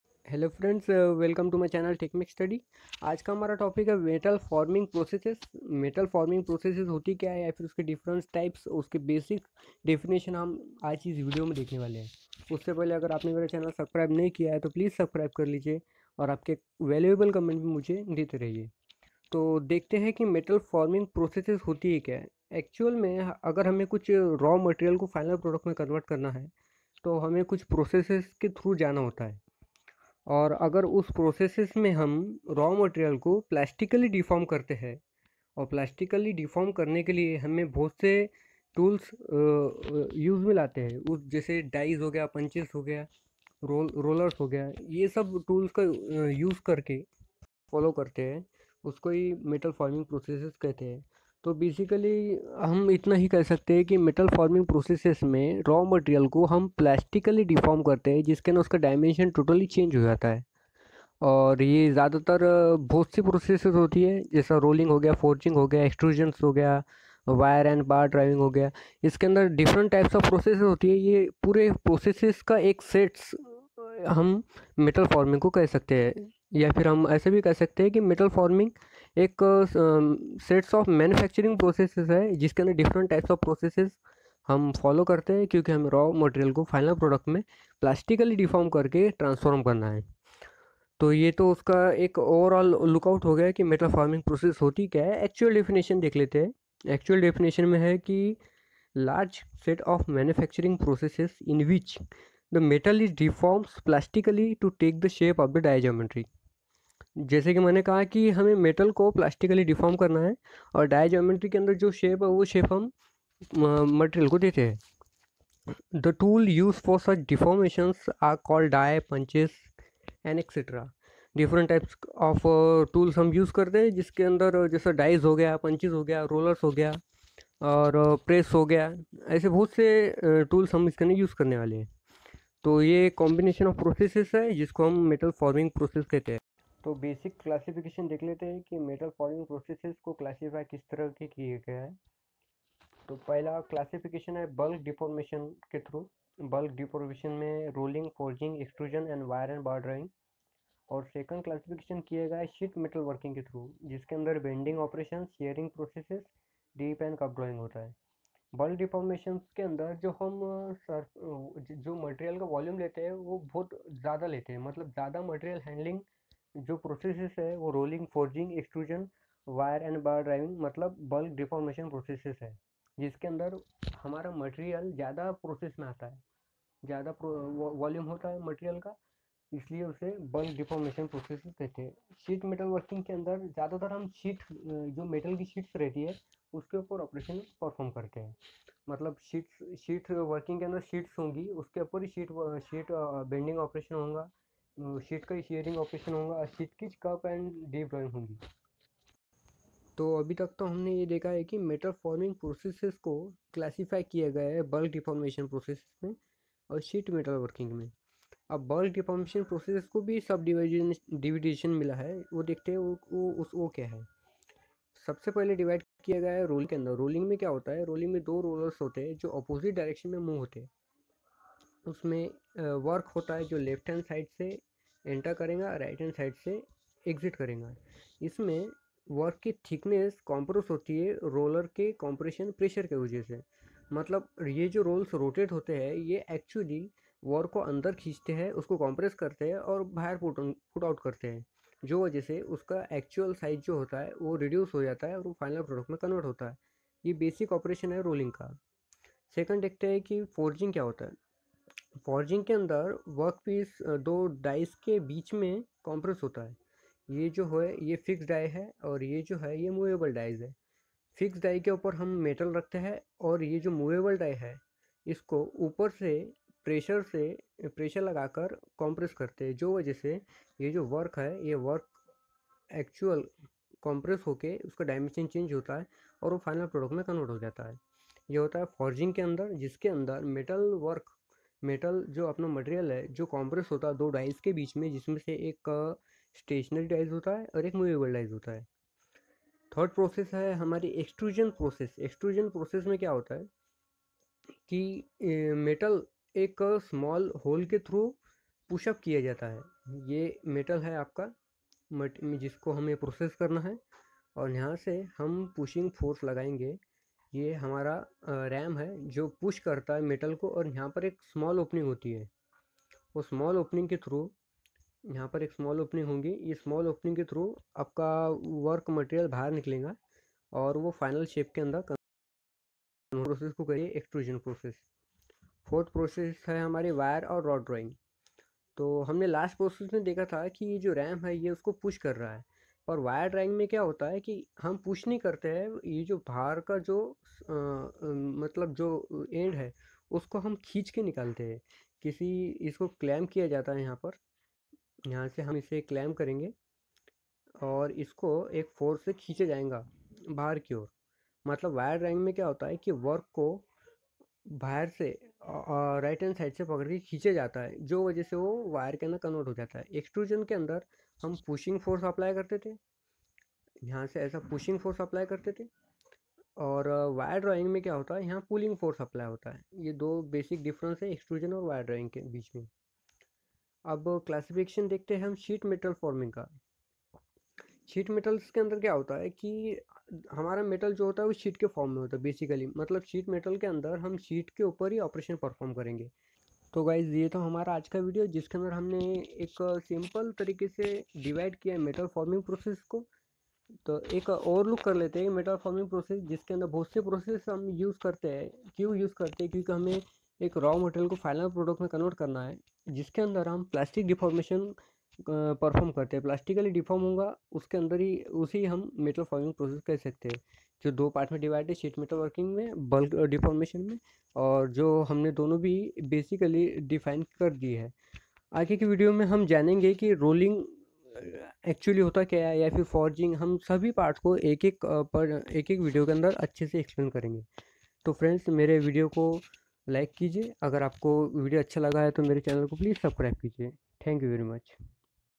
Metal Forming Process , Types in Hindi
Basic description of FORMING PROCESS , Types and Defination .